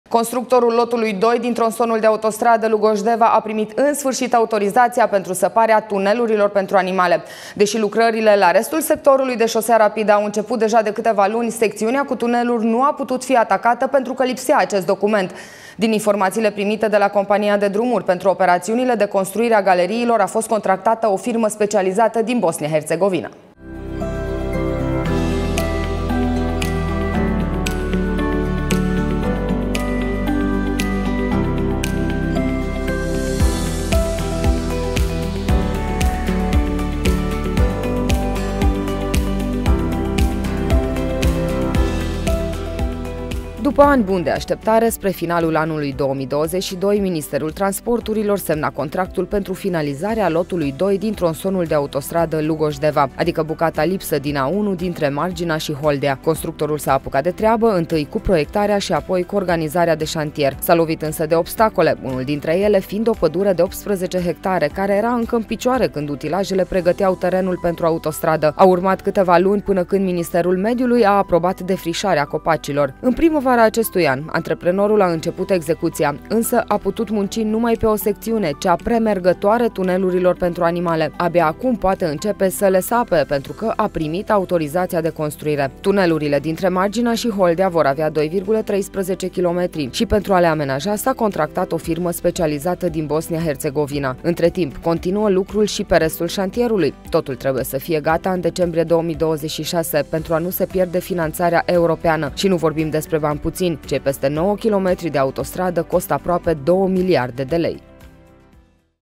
Se pot săpa tunelurile pentru animale
Tunelurile de pe lotul II din Lugoj - Deva, dintre Margina și Holdea, vor avea 2,13 kilometri și pentru a le construi s-a conttractat o firmă specializată din Bosnia Herțegovina. Între timp, continuă lucrul și pe restul șantierului. Totul trebuie să fie gata în decembrie 2026 pentru a nu se pierde finanțarea europeană.